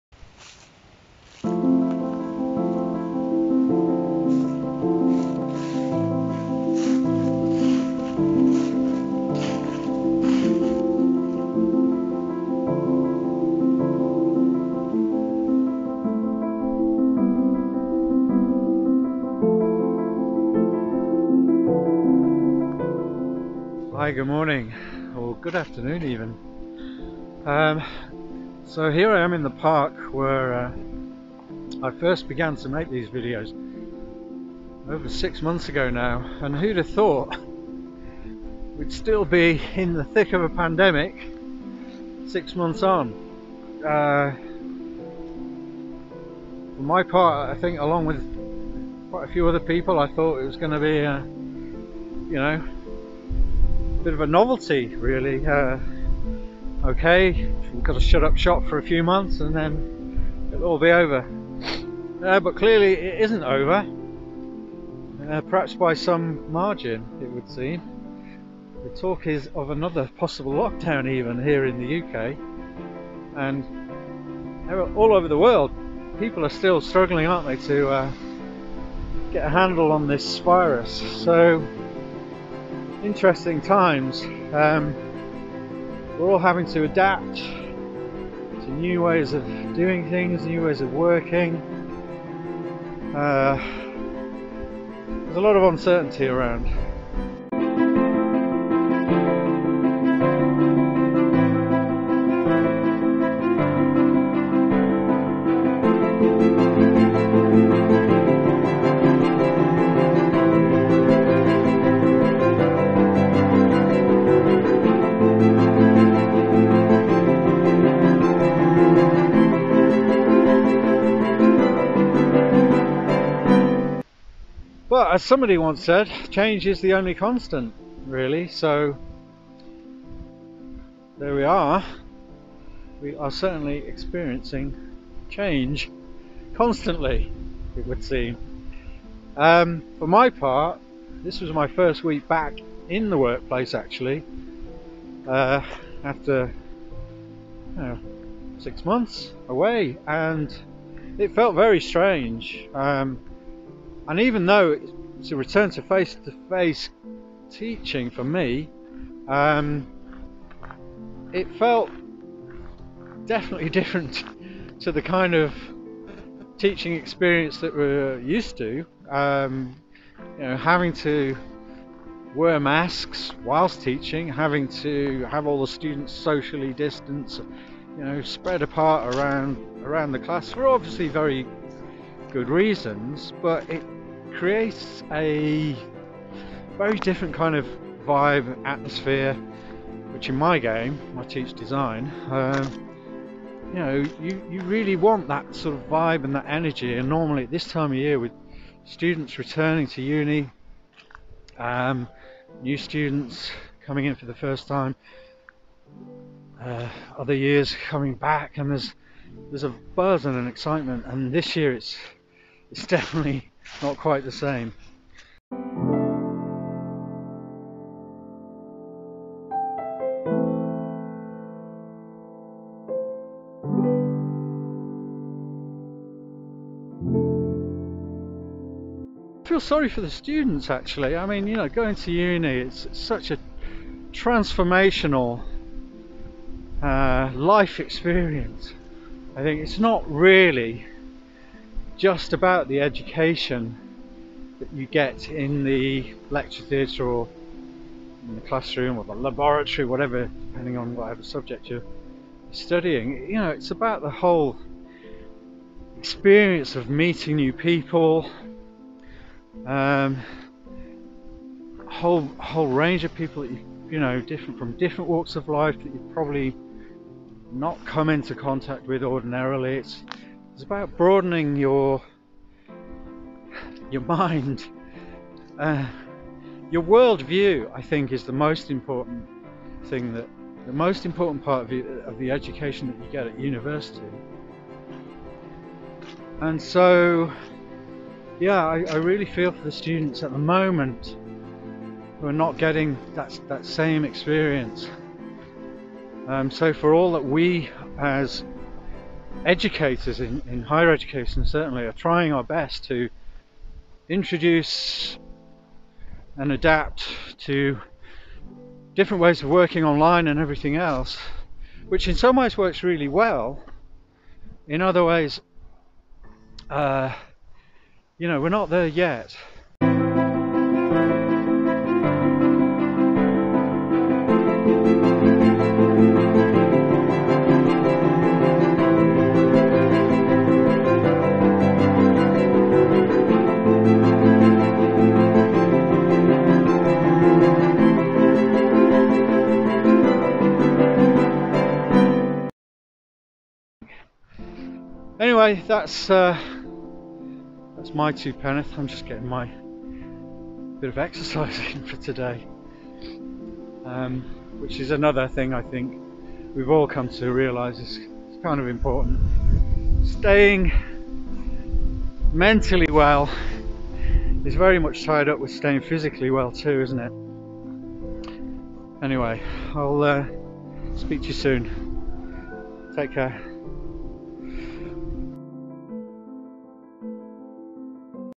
Education
A return to my lockdown Vlog-Thoughts on the changing nature of Education in the Covid 19 Pandemic